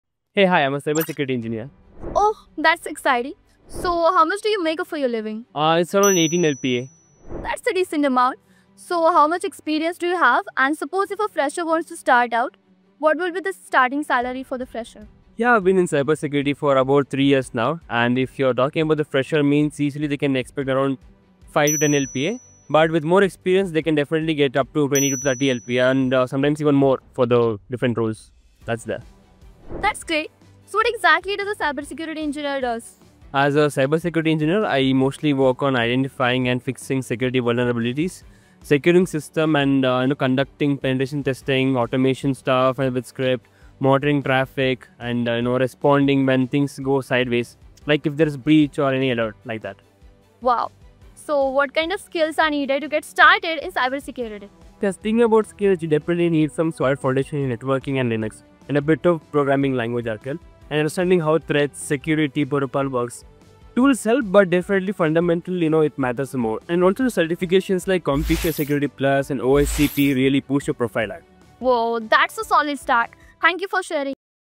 How much does a Cyber Security Engineer make? | Cyber Security Engineer Salary in 2025 | Intellipaat
How much does a Cyber Security Engineer make? | Cyber Security Engineer Salary in 2025

Wondering how much a Cyber Security Engineer makes in 2025? 💼 This video by Intellipaat dives into the cyber security salary trends and gives you real insights into the earning potential in a cyber security career—both in India and globally. Whether you're just starting out or planning a switch, understanding how much do cyber security engineer make is crucial to planning your future.
As cybersecurity careers continue to grow, the demand for skilled professionals is at an all-time high. From entry-level roles to senior cyber security engineer positions, this video explores everything you need to know about a career in cybersecurity. Learn what influences your pay scale, what skills companies are looking for, and how a cyber security course or certification can boost your cyber security career and take it to the next level.

FAQs:
🔹 How much do cyber security engineer make in India in 2025?
 A cyber security engineer in India can earn anywhere from ₹6 LPA at entry level to ₹25+ LPA with experience and certifications. The field of cyber security is booming, and pay scales are only increasing.
🔹 Is a cyber security career worth it in 2025?
 Absolutely. A cyber security career offers excellent pay, high demand, and long-term job security. It’s one of the fastest-growing domains in tech today.
🔹 What are the most in-demand skills for a career in cybersecurity?
 Top cybersecurity careers require skills in network security, ethical hacking, cloud security, risk analysis, and compliance. Tools like Wireshark, Kali Linux, and Splunk are commonly used.